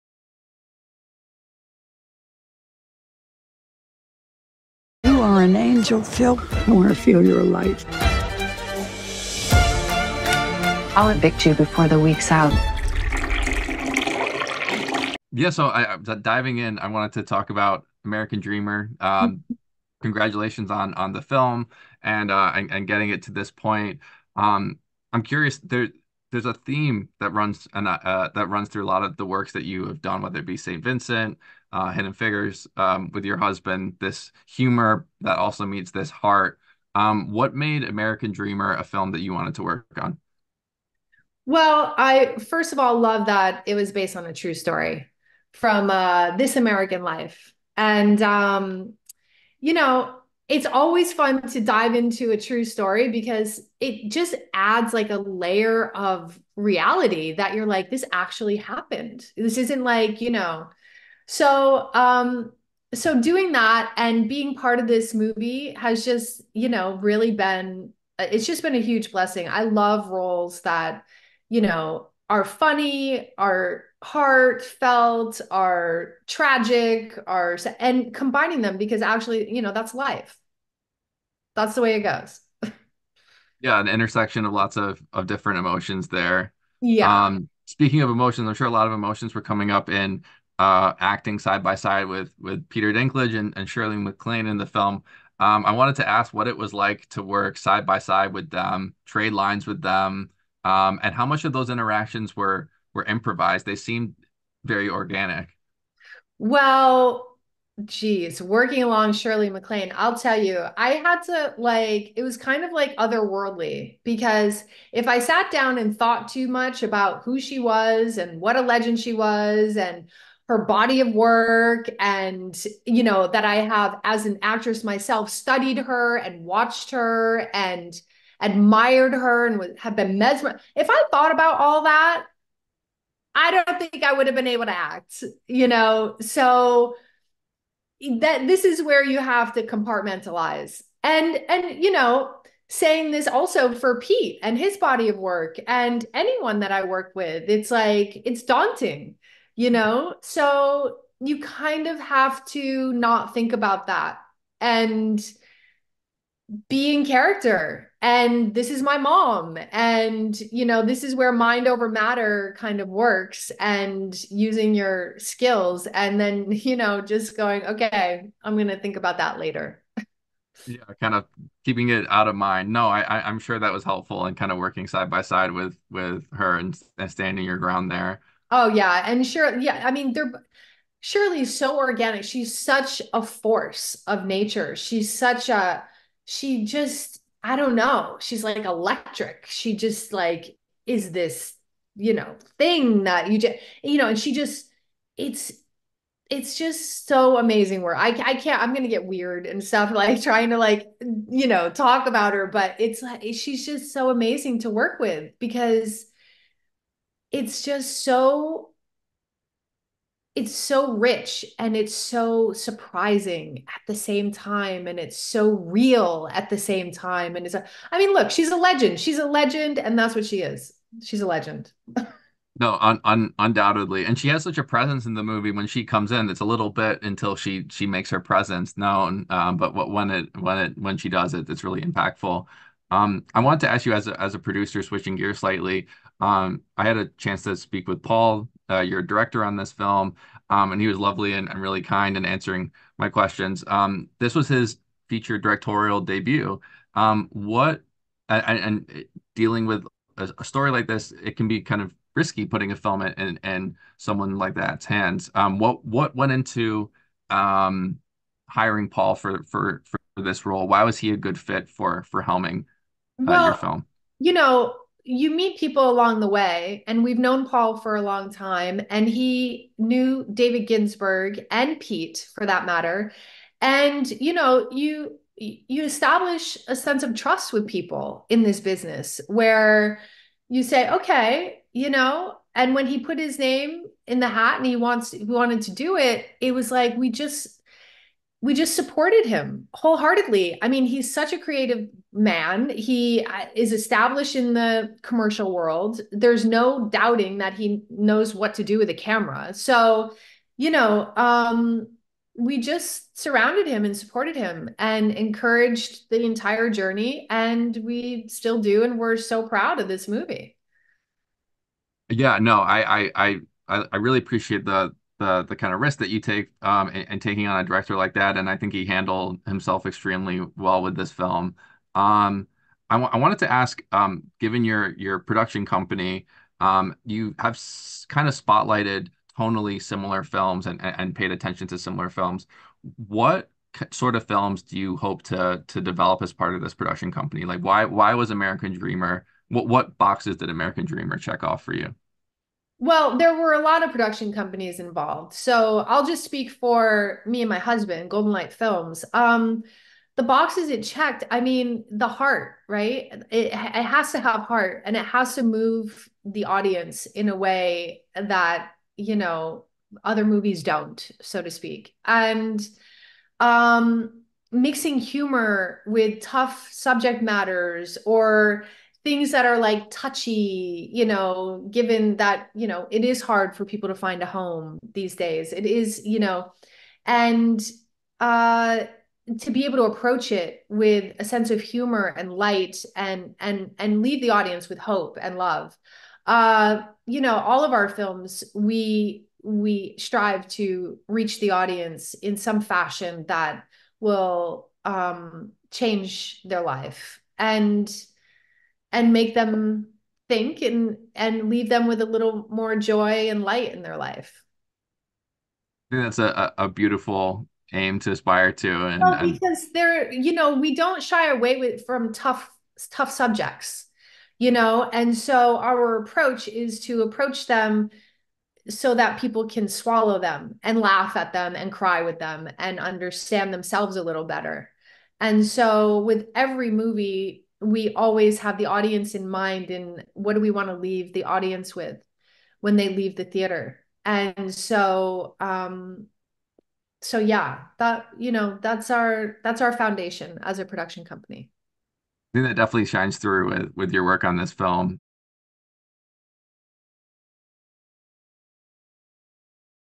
Exclusive Interview: American Dreamer Star + Producer Kim Quinn Talks New Comedy
We talk with American Dreamer producer and actor to discuss the new Peter Dinklage-led film.